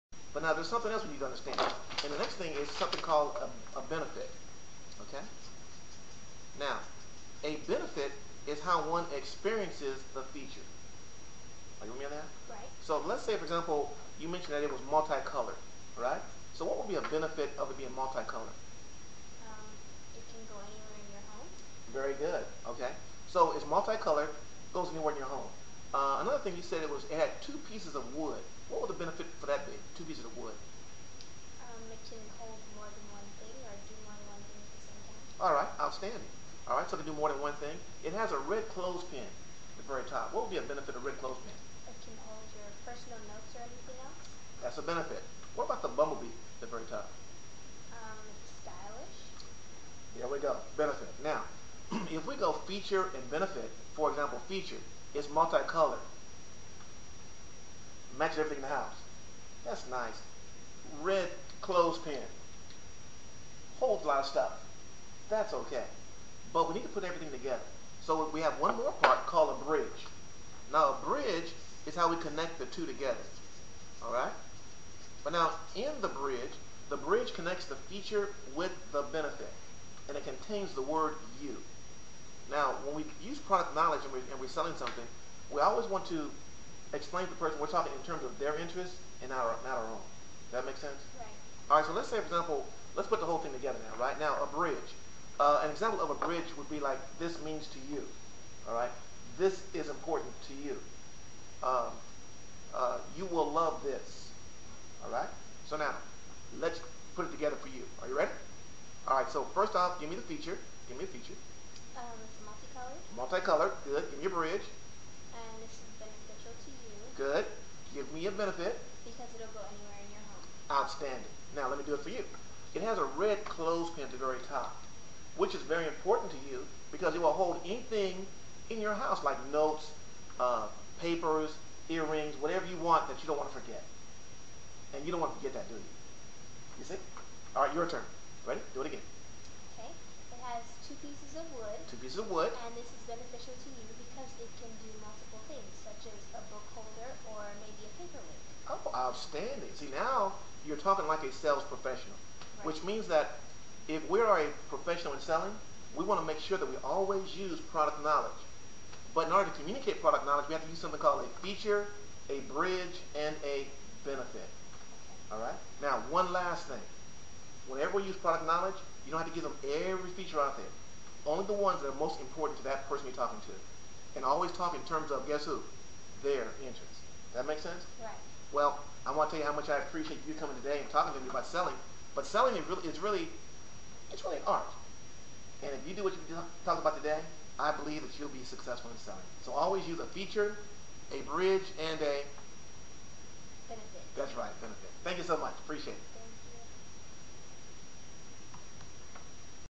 Nate Selling Skills part 2
Selling skills using product knowledge talking in terms customers
understand part 2